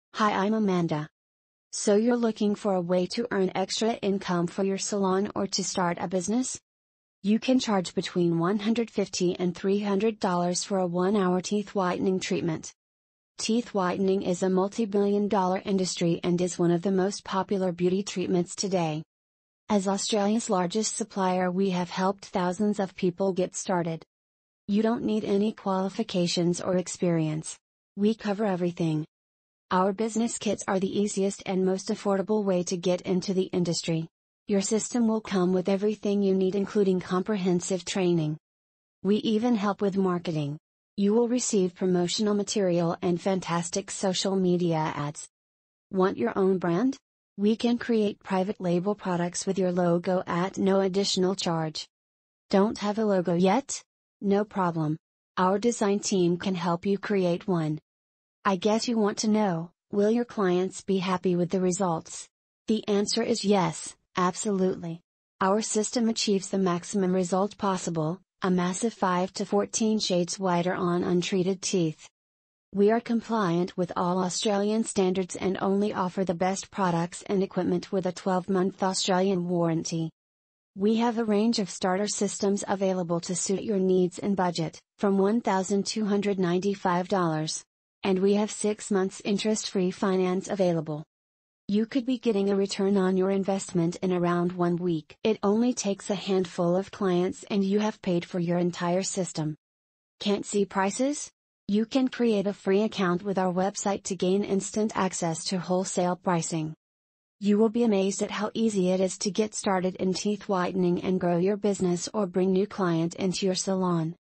How to get started in teeth whitening
Teeth Whitening is a multi-billion dollar industry and is one of the most popular beauty treatments today. By offering teeth whitening as a service, you'll have a product people are already crazy about.

Perfect as a stand-alone business from home or mobile or a perfect addition to a salon.  Get extra sales from existing clients as well as bringing new clients.

✅ WANT TO MAKE MASSIVE PROFITS?

Earn $149-$299 per treatment which takes less than 1 hour!

 ✅ HOW MUCH DOES IT COST TO GET STARTED?

You can have your own profitable teeth whitening business from $1,295.  We have 6 months interest-free finance available.

When offering teeth whitening, you’ll be offering a service that is in high demand, has massive profit margins, low overheads, and low start-up costs.

1.  Low start-up cost

2.  Work the hours you want

3.  All equipment supplied to get you started

4.  Complete training supplied

5.  Excellent backup support

6.  Huge demand

7.  NO hidden cost

8.  NO ongoing fees

9.  NO percentage to pay

10. You keep all your profits and the equity value you create in your business!